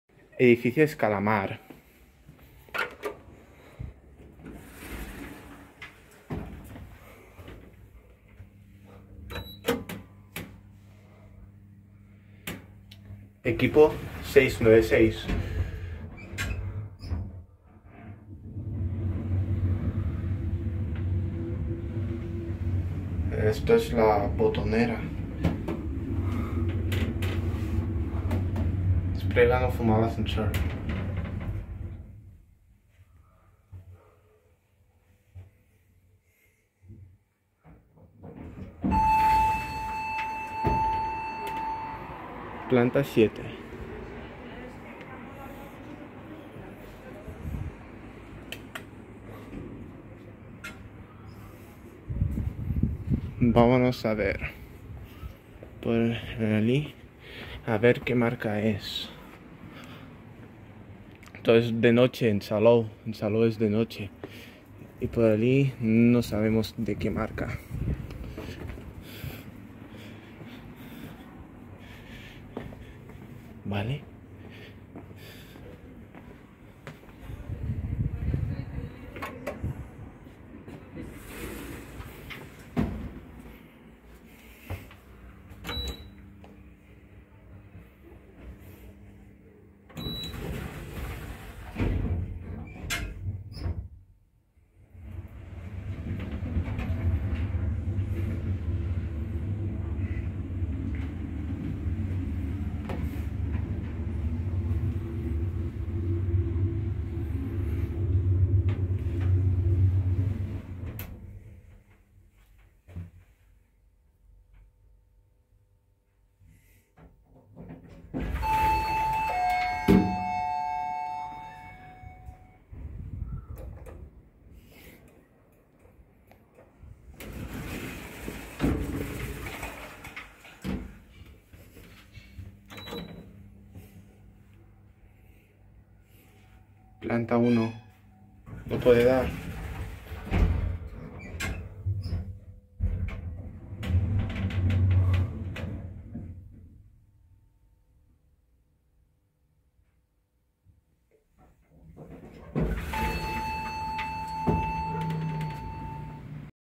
Ayssa lift in a building Scala Mar
Location: Carrer Tortosa, 18, Salou, Tarragona
Capacity: 300 kg 4 pers
Serial num: RAE 696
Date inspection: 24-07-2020
Cabin lift: Grey
Floors: 1, 2, 3, 4, 5, 6, 7
Doors: A, B, C, D, E
2nd floor: Exit
Name's building: Scala Mar